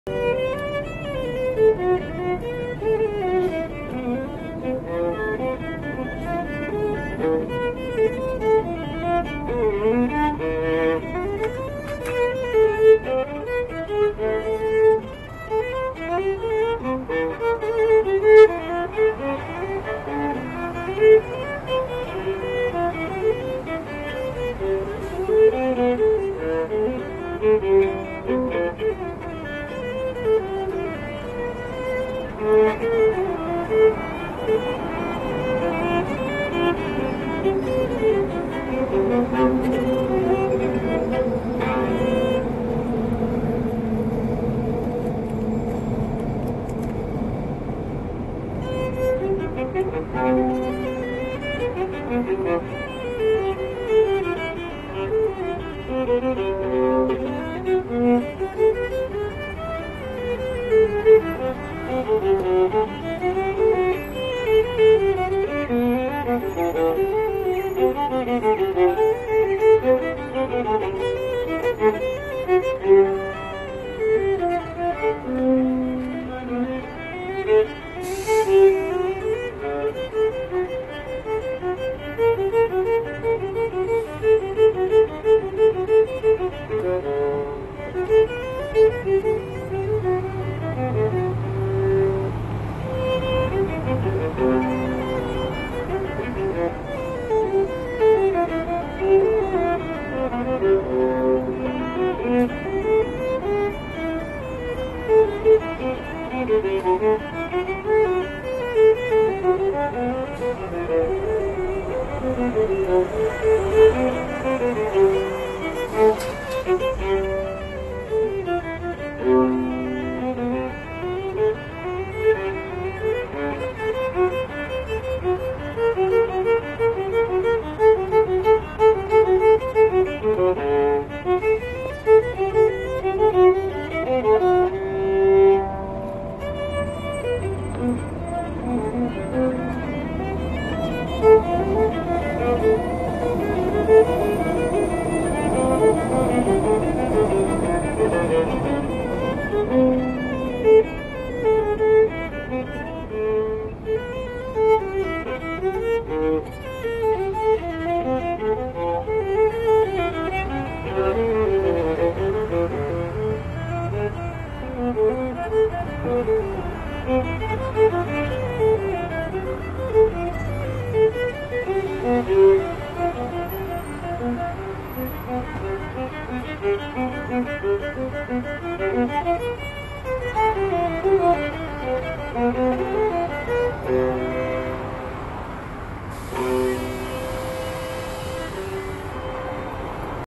Ezra Azmon Viola Player In Vancouver BC 2016 360 Video for Oculus, Daydream, GearVR -
Ezra Azmon is a Viola Player and he plays very well.  We were fortunate to catch him at the  Broadway Commercial Skytrain Busker April 2016  In Vancouver BC 2016  on 360 Video with the Ricoh ThetaS 360 Video for Oculus, Daydream, GearVR -